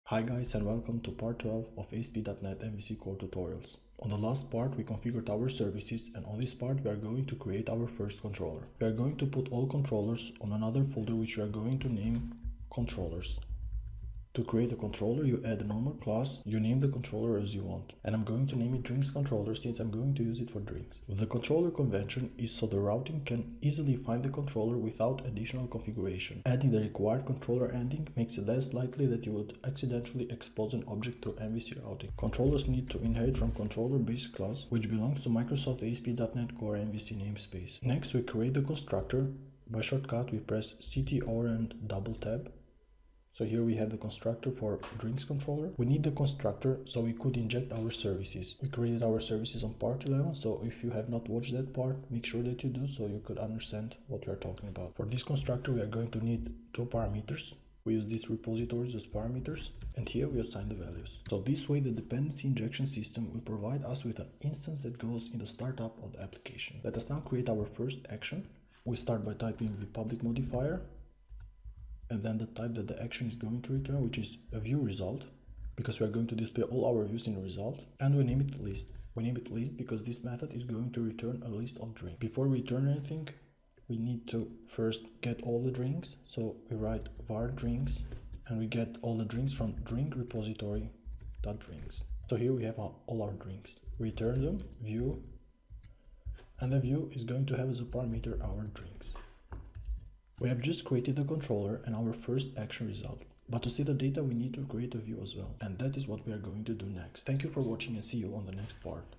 12. FIRST CONTROLLER - Asp.Net CORE MVC
On this part you are going to learn how to add the first controller to your ASP.NET CORE MVC application.